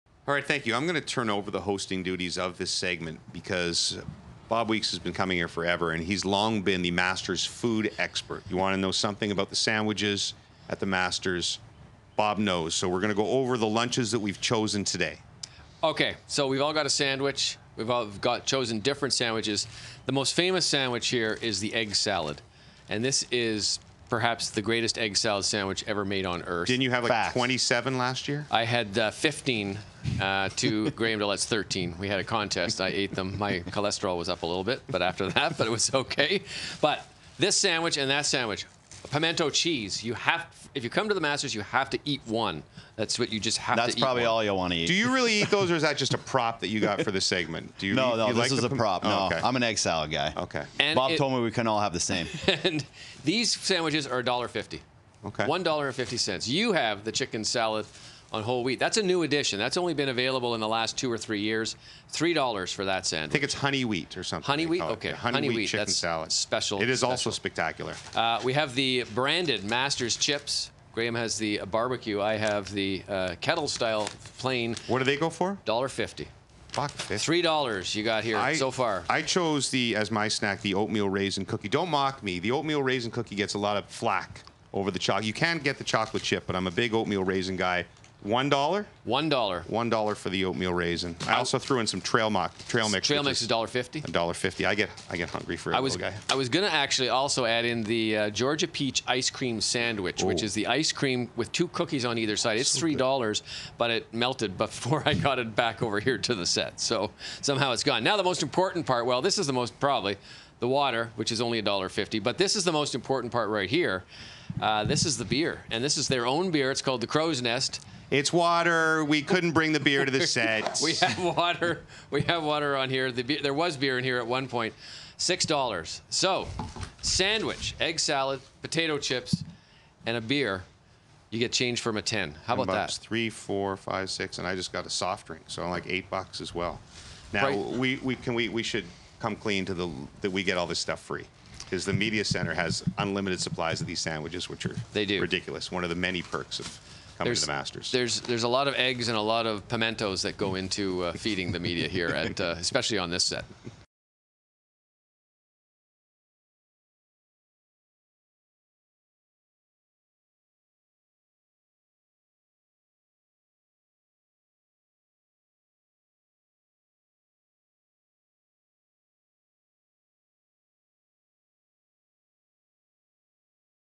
James, Graham and Bob go over the incredible food menu at The Masters
While we have memories of amazing moments from The Masters at Augusta National, those in attendance also get to experience the incredibly priced food selection. TSN's James Duthie, Graham DeLaet and Bob Weeks break it all down from $1.50 sandwiches to $6.00 beers.